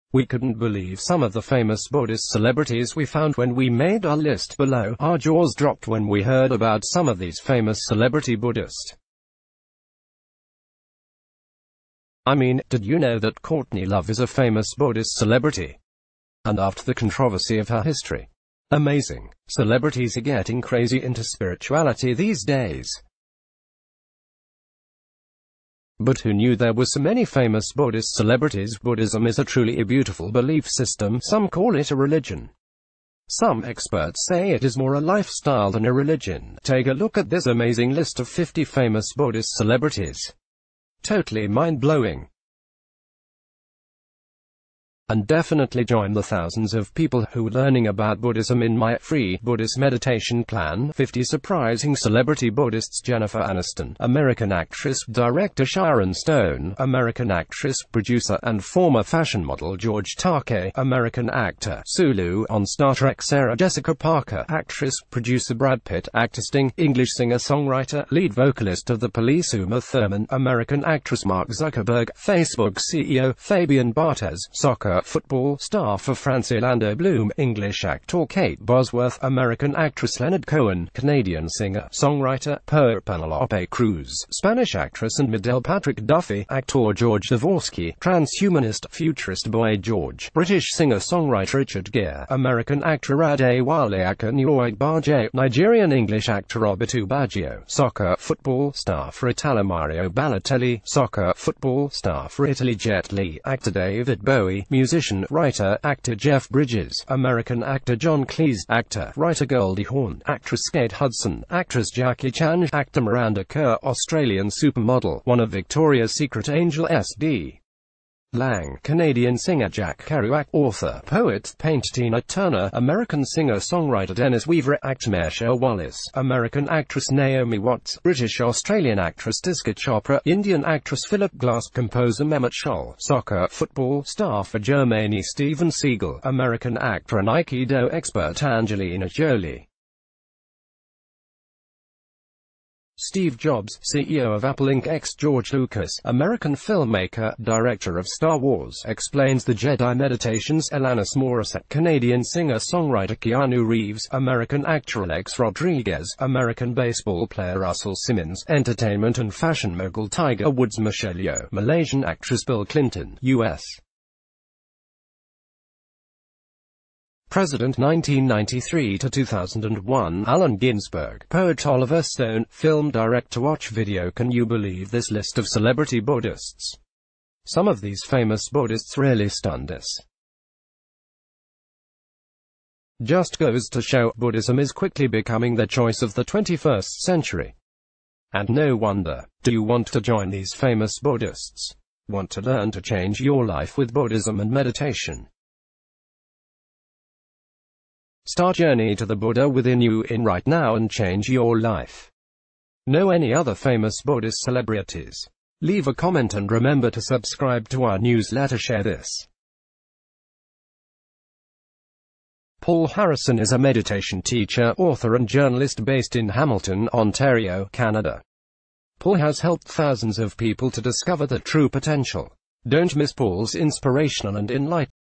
50 Famous Buddhist Celebrities You'll Be Shocked To See [LIST] | BuzzFresh News
50 Famous Buddhist Celebrities You'll Be Shocked To See [LIST] | BuzzFresh News
We couldn’t believe some of the famous Buddhist celebrities we found when we made our list (below). ...